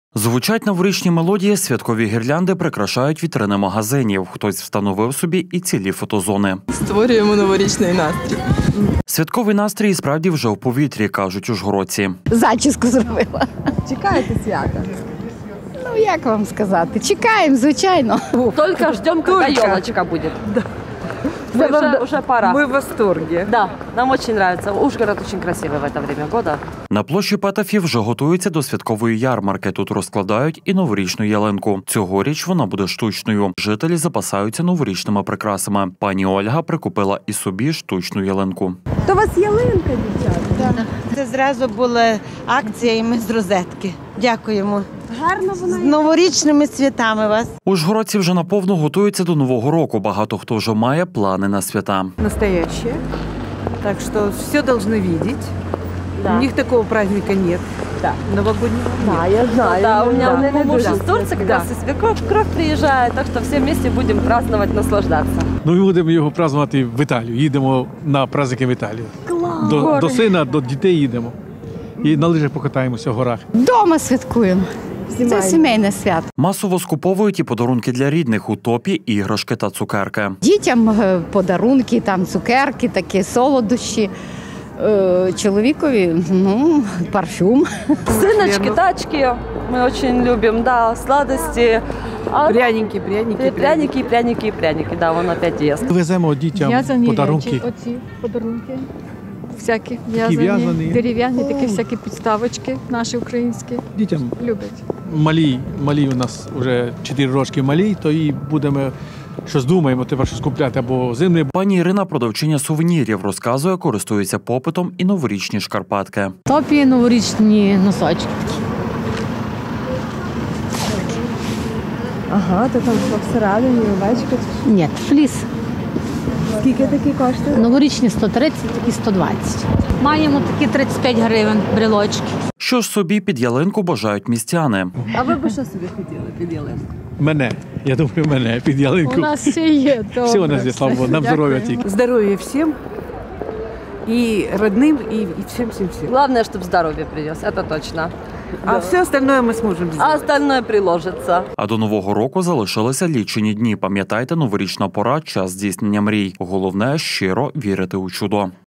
Свято наближається
Свято наближається. До нового року залишилися лічені дні. В Ужгороді вже на повну готуються до свята. Тут вже майже готова новорічна ялинка. Скуповуються до свят і містяни. Звучать новорічні мелодії, святкові гірлянди прикрашають вітрини магазинів, хтось встановив собі й цілі фотозони. Святковий настрій і справді вже у повітрі, кажуть ужгородці. На площі Петефі вже готуються до святкової ярмарки, тут розкладають і новорічну ялинку, цього річ вона буде в штучна. Вже і жителі запасаються новорічними прикрасами. Пані Ольга прикупила і собі штучку ялинку. Ужгородці вже на повну готуються до Нового року. Багато хто вже має плани на свята. Масово скуповують і подарунки для рідних. У топі іграшки та цукерки. Пані Ірина продавчиня  сувенірів, розказує користуються попитом і новорічні шкарпетки. Що ж собі під ялинку бажають містяни?А до нового року залишилися лічені дні. Пам’ятайте, новорічна пора – час здійснення мрій. Головне щиро вірити у чудо.